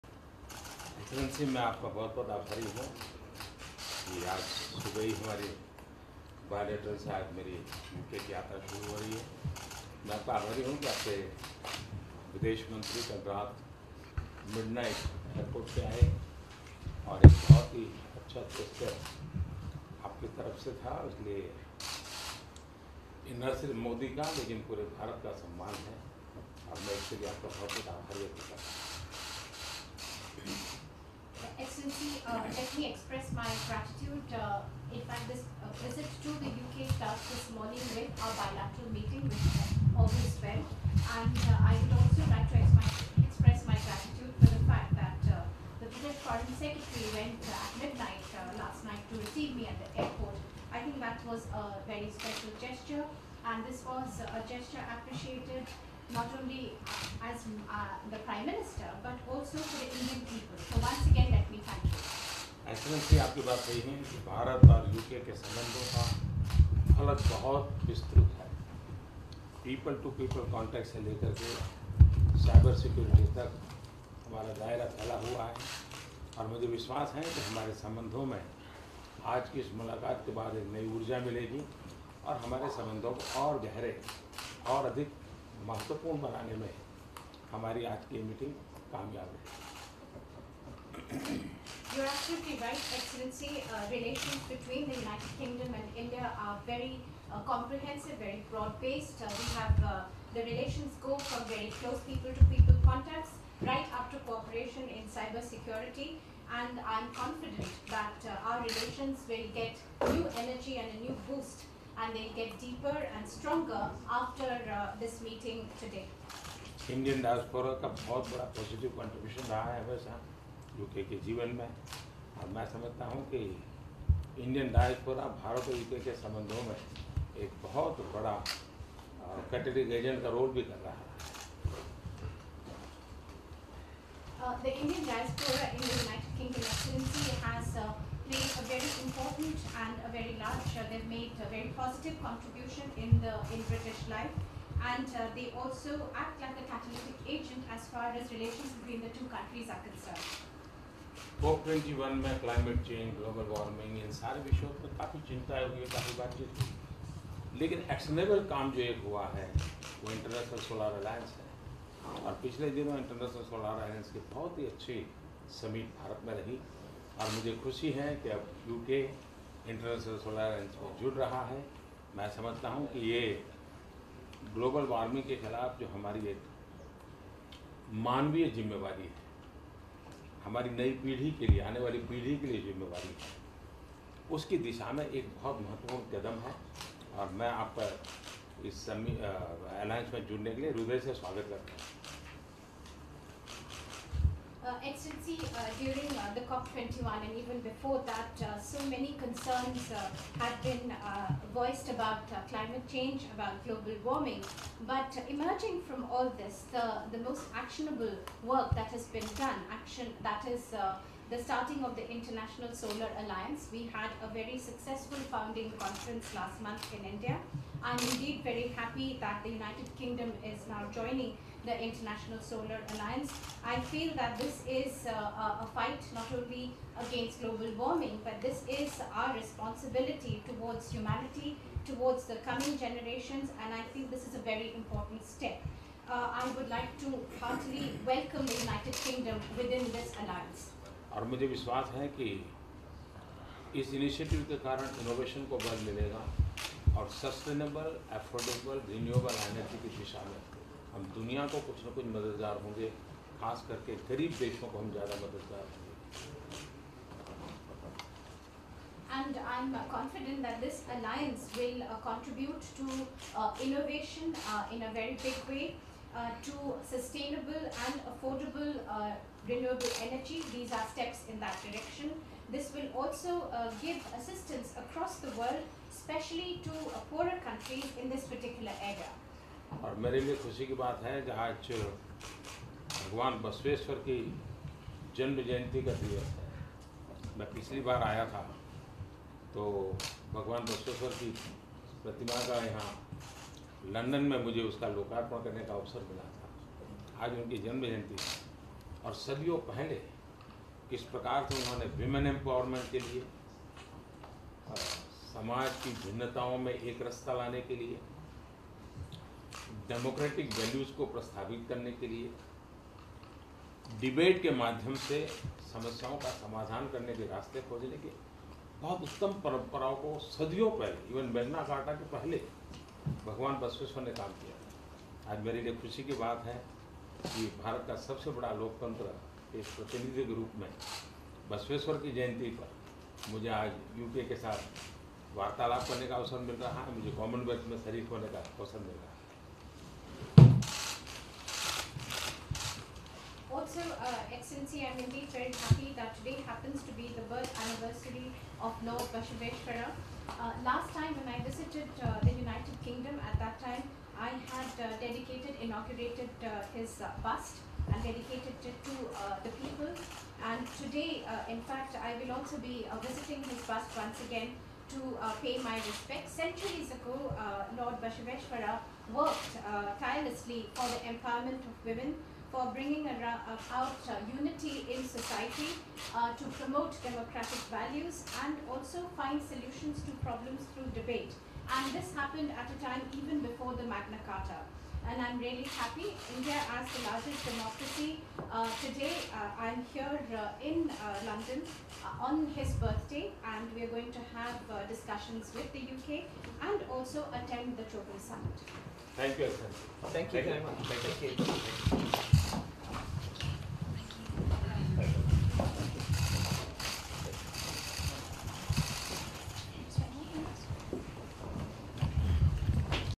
PM Modi meets UK Prime Minister Theresa May
Prime Minister Narendra Modi meets UK Prime Minister Theresa May